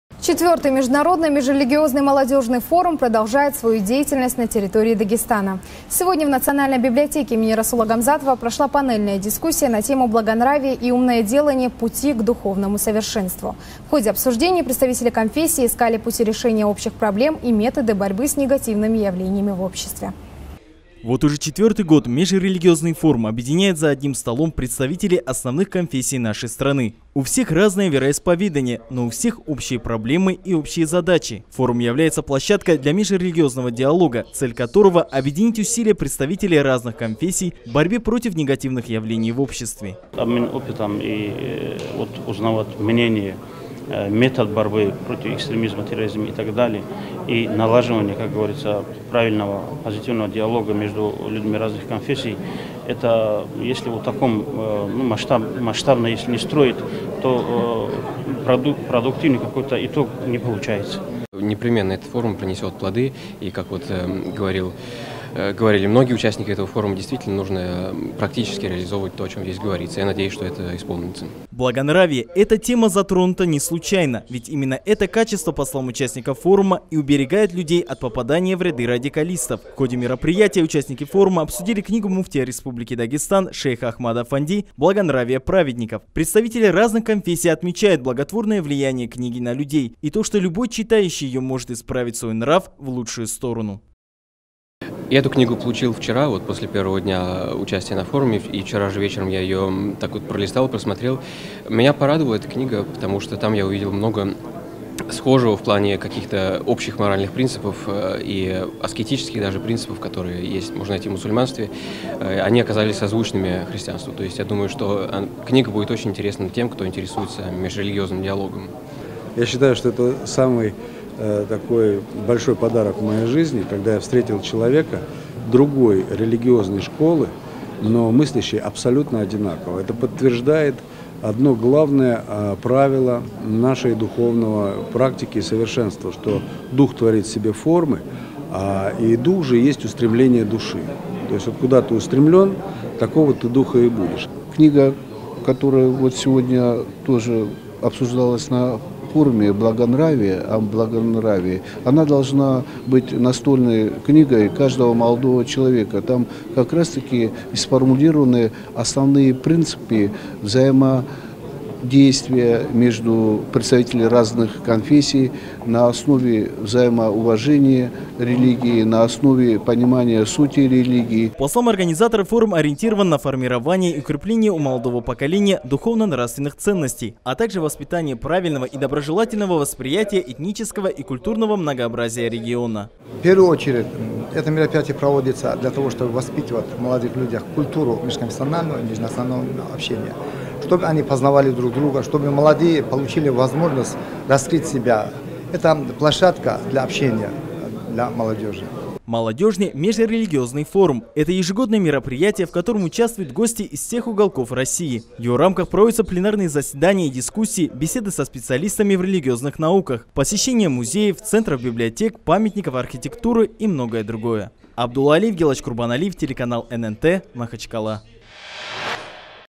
Благонравие - путь к духовному совершенству
В рамках четвертого международного межрелигиозного молодежного форума в национальной библиотеке им. Расула Гамзатова прошла панельная дискуссия религиозных деятелей «Благонравие и умное делание: пути к духовному совершенству».
По мнению всех участников форума именно благонравие уберегает человека от негативных явлений